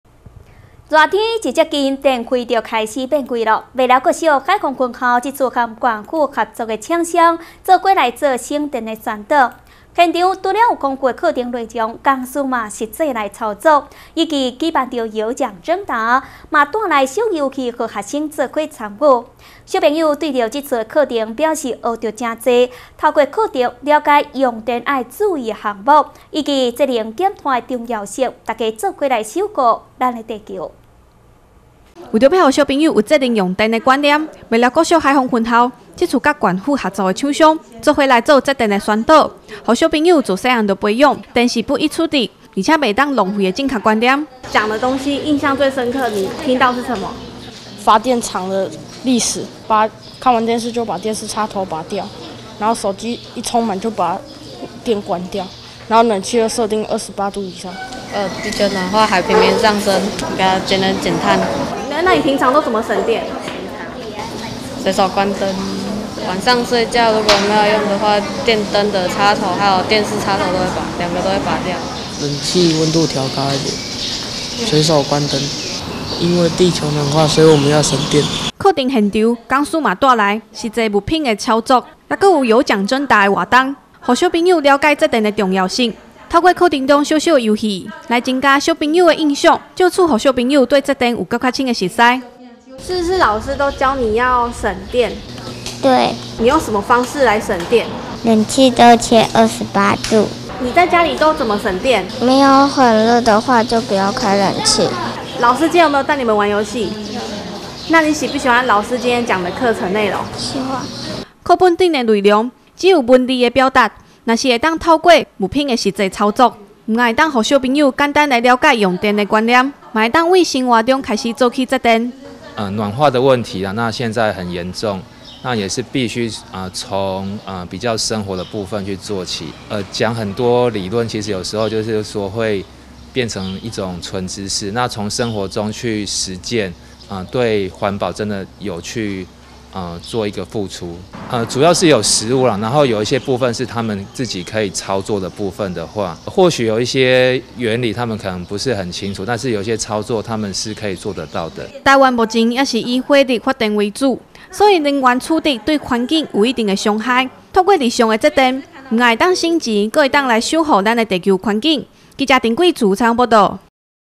雲林新聞網-分校宣導節電觀念 實物操作增加印象
夏天一接近電費就開始變貴，麥寮國小海豐分校這次跟縣府合作的廠商，一起來做節電的宣導，現場除了有豐富的課程內容講師也實際操作，以及(有獎徵答)，也帶來小遊戲來給學生做伙參與。小朋友也對這次課程表示學到很多，透過課程了解用電要注意，以及要(節能減碳)，一起來守護我們的地球。